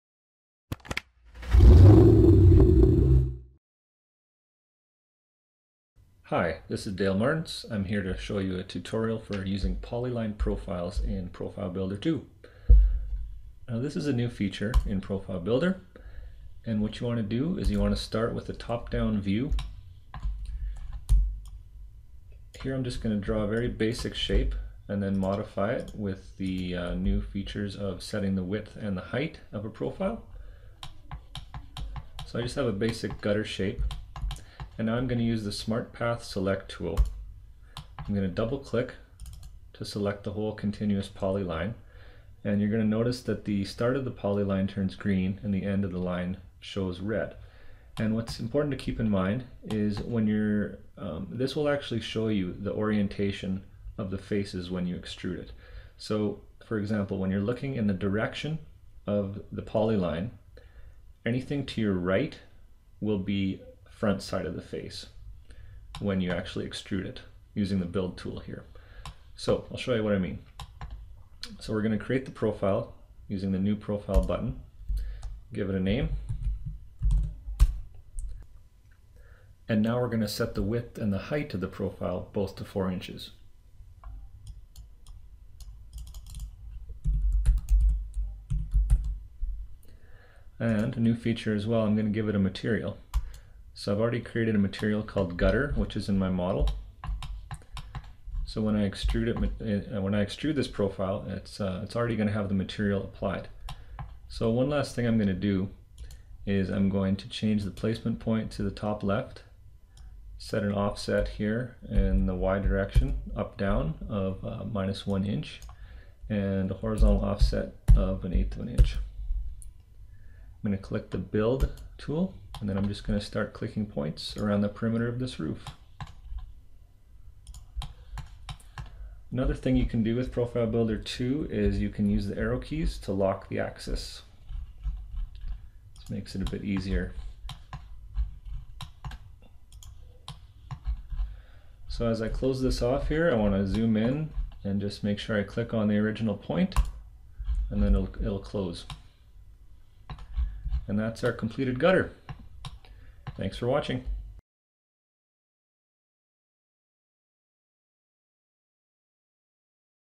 Profile Builder 2 - Polyline Profiles
A demonstration about how to create a polyline profile in the SketchUp Extension Profile Builder 2.